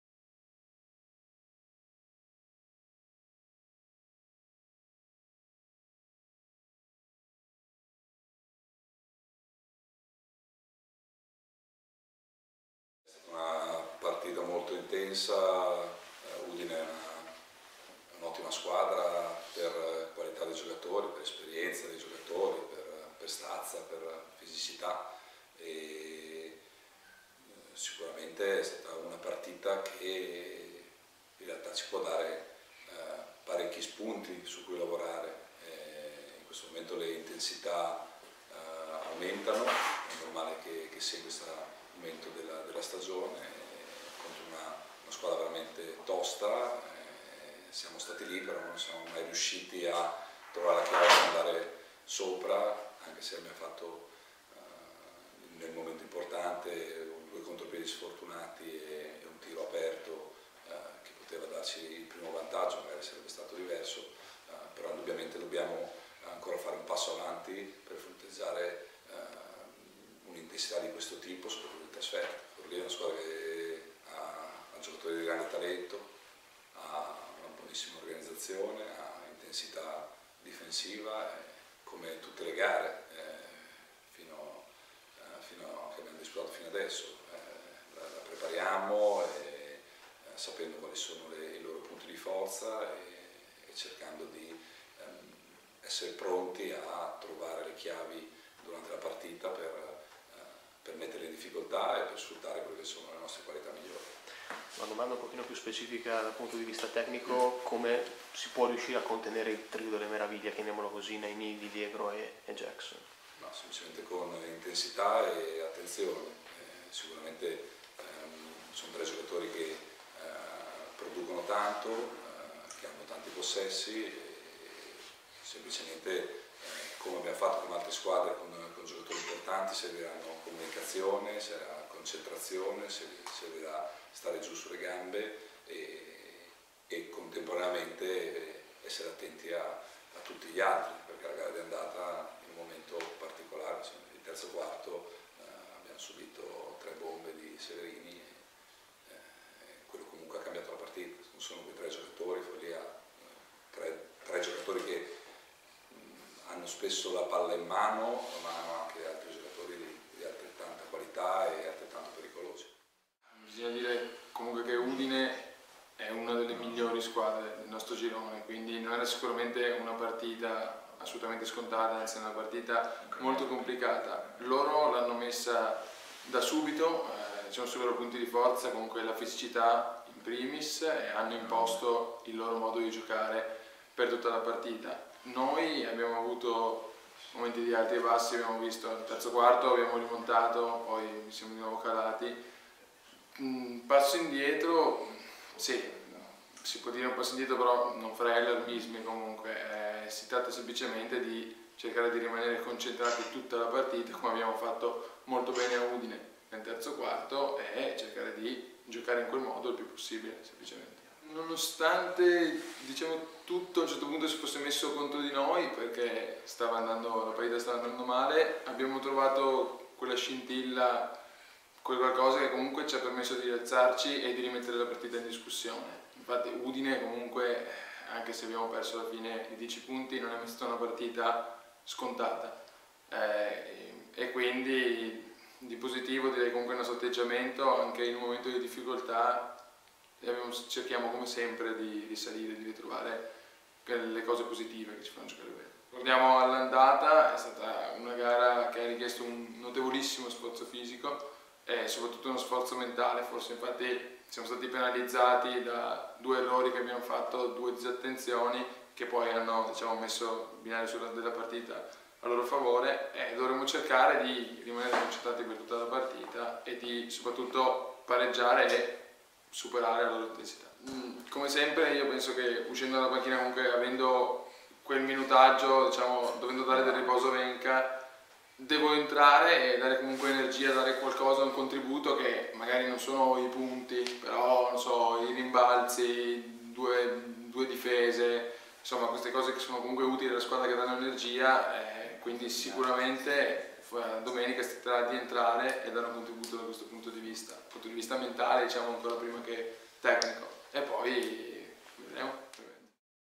Pre-partita di Stings Mantova - Forlì con coach Lamma e Matteo Ferrara
In vista della gara con l'Unieuro Forlì, abbiamo raccolto le dichiarazioni rilasciate dal coach della Dinamica Generale Mantova Davide Lamma e dal playmaker Matteo Ferrara.